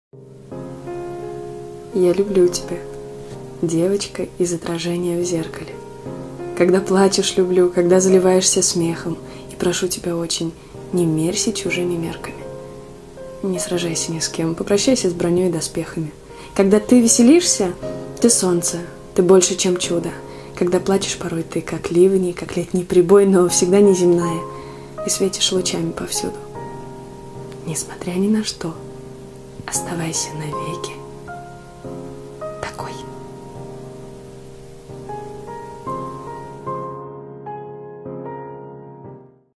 я люблю с тебя | Читает автор: Логинова Валя
я люблю тебя, девочка из отражения в зеркале.
когда плачешь люблю, когда заливаешься смехом.
и прошу тебя очень: не меряйся чужими мерками,
не сражайся ни с кем, попрощайся с броней и доспехами.

когда ты веселишься - ты солнце, ты больше, чем чудо.
когда больно порой, ты как ливни, как летний прибой,
но всегда неземная, и светишь лучами повсюду.
несмотря ни на что, оставайся навеки такой.